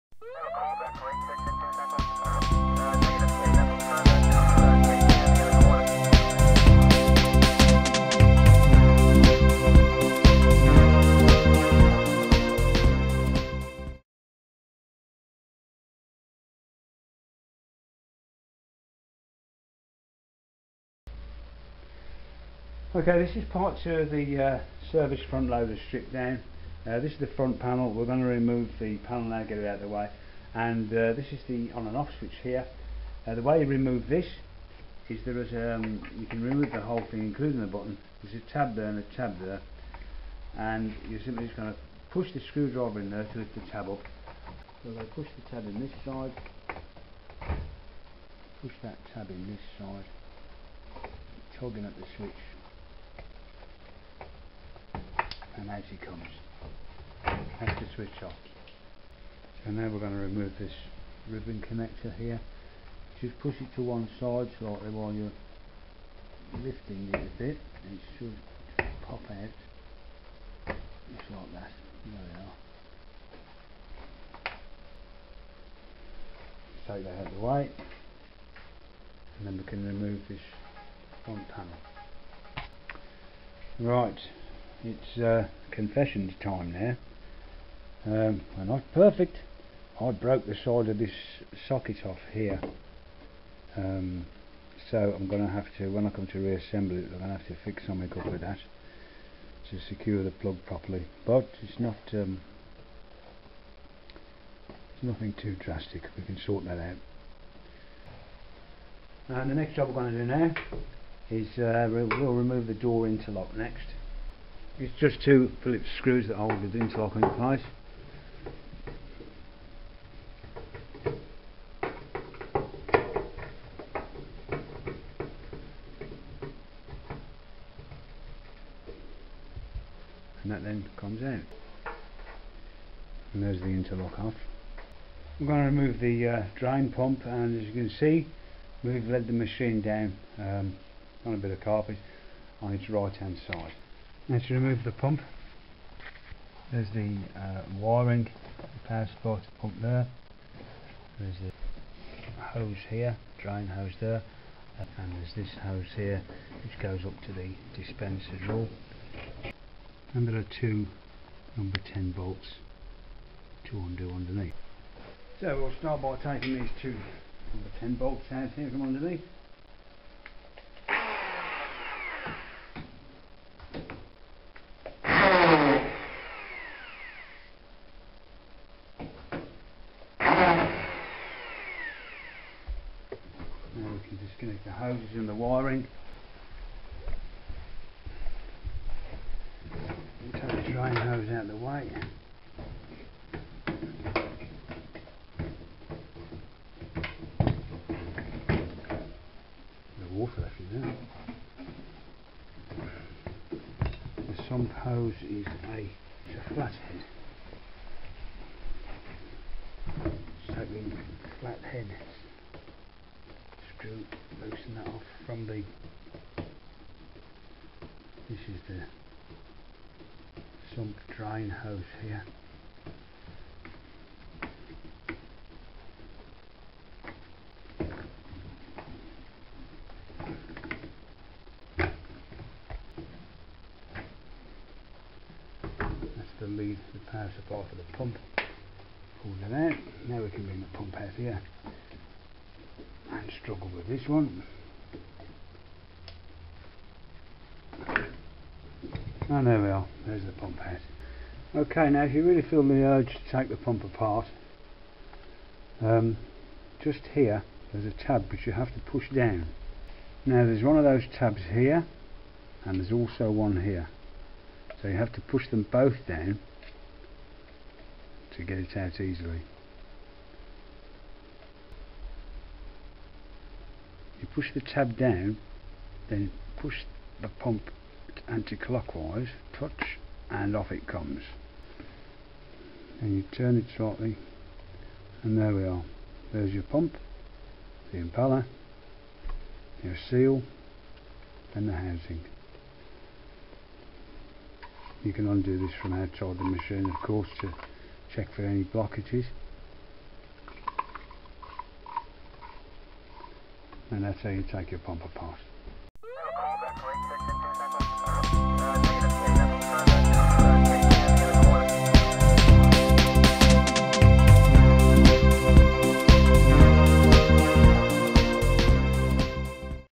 Servis front loader stripdown part 2
servis front loader stripdown part 2. Front panel, interlock and pump.